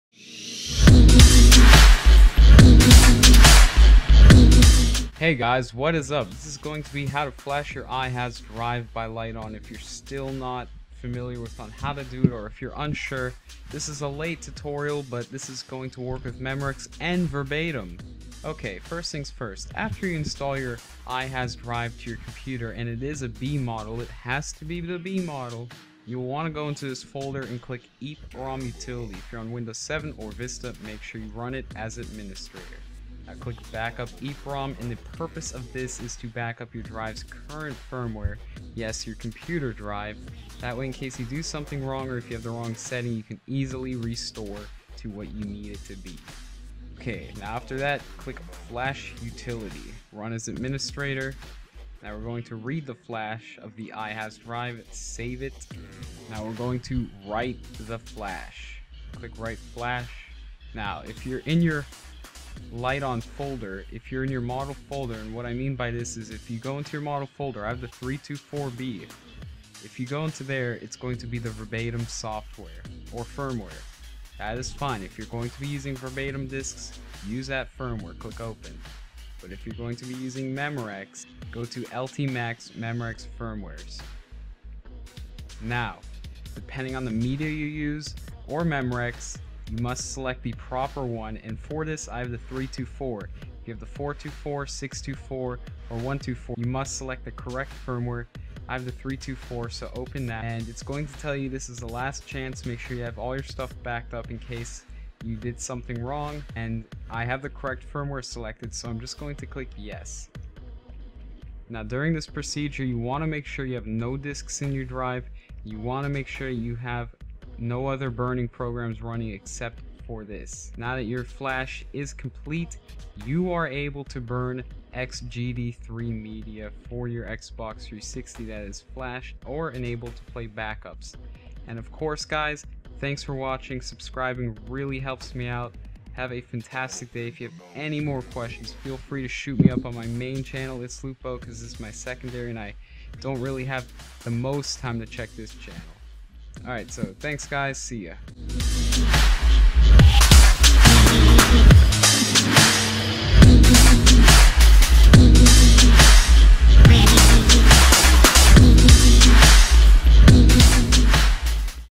How to flash Liteon iHas drive to burn XGD3 | Tutorial for flashing B series iHas drives
Alright pretty simple tutorial on how to flash your liteon iHas drive you do need a computer with a SATA port to use this drive for burning of Xbox 360 Game disk 3 media.

[WARNING] YOU NEED THIS DRIVE TO SAFELY PLAY AND BURN XGD3 GAMES.

The drives covered in this are
124B
224B
324B
424B
524B
624B